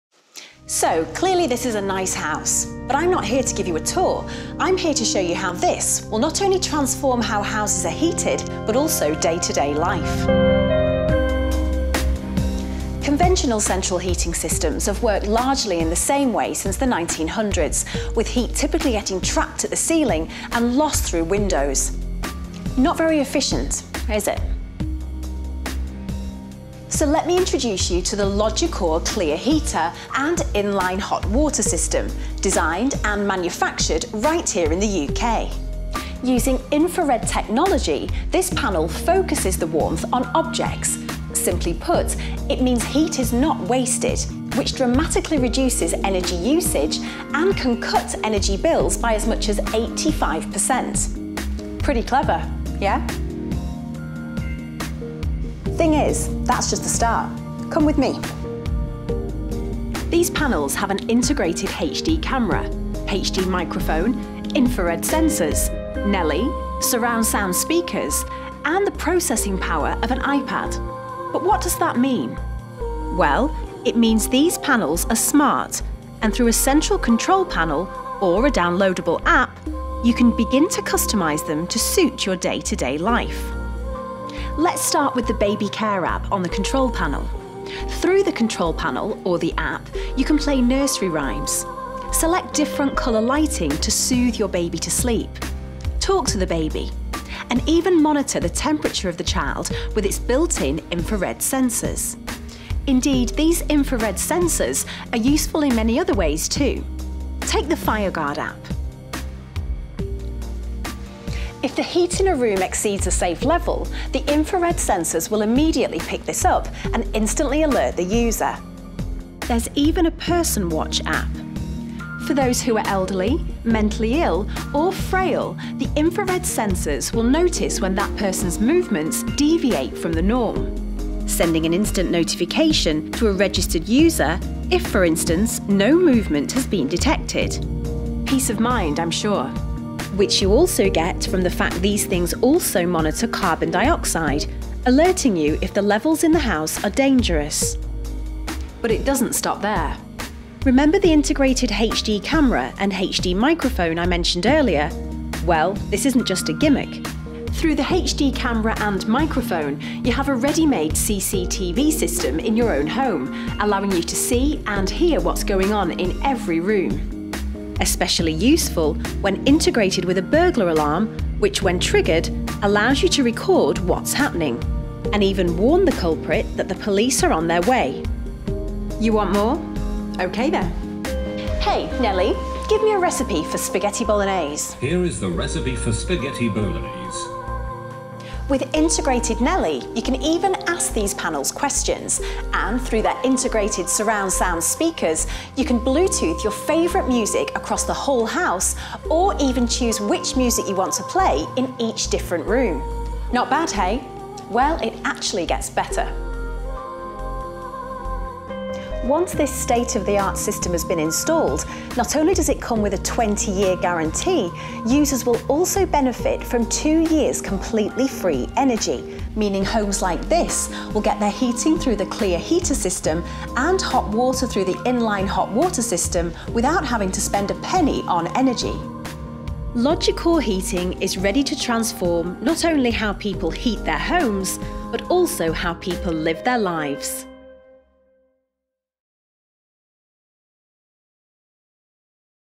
Logicor Explainer video
This video forms the basis of our TV advert. Here you can see it as a full length short film. You will see the 2nd generation Logicor Clear Heaters in action plus our InLine Hot Water System which will give you an idea what they can look like in your home. It will also show you our Application Services in action. Anyone with our products can have access to some or all of these services. It's another step forward showing Logicor is keeping one eye on the future whilst recognising it needs to bring innovative products to the market today.